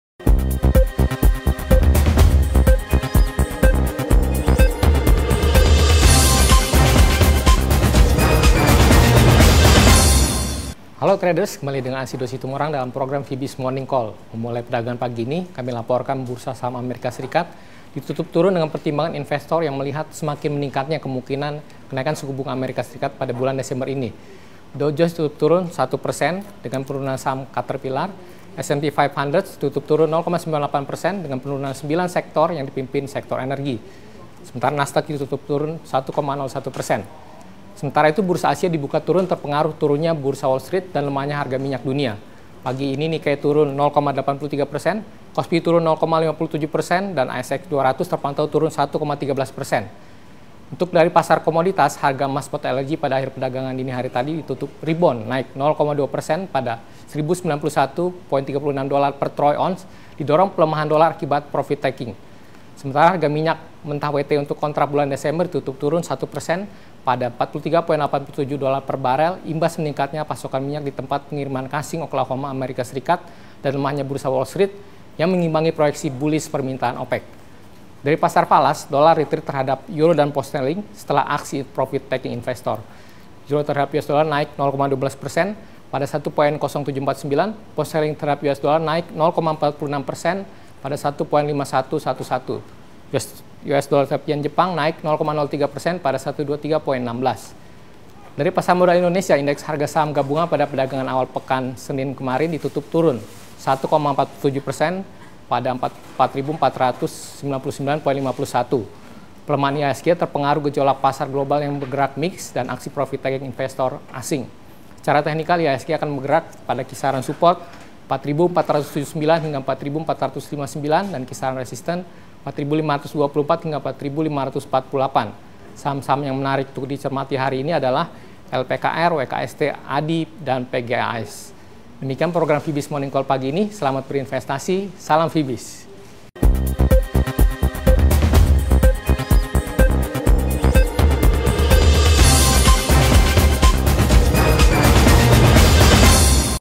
Dollar Retreat, Harga Emas Naik Tipis, Vibiznews 10 November 2015
Program Vibiz Morning Call by Vibiznews, 10 November 2015

Memulai perdagangan pagi ini, kami laporkan Bursa Saham AS ditutup turun dengan pertimbangan investor yang melihat semakin meningkatnya kemungkinan kenaikan suku bunga AS bulan Desember ini. Dow Jones ditutup turun 1 %, di 17,730.4, dengan penurunan saham Caterpillar. S & P 500 ditutup turun 0,98 %, pada 2,078.58, dengan penurunan sembilan sektor yang dipimpin sektor energy. Nasdaq ditutup turun 1,01 %, di 5,095.3.

Sementara itu, bursa Asia dibuka turun terpengaruh turunnya bursa Wall Street dan lemahnya harga minyak dunia. Pagi ini Nikkei turun 0,83% di 19479.72. Kospi turun 0,57% di 2014.14. Sementara terpantau ASX 200 turun 1,13% di 5061.80.

AS/VBN/VMN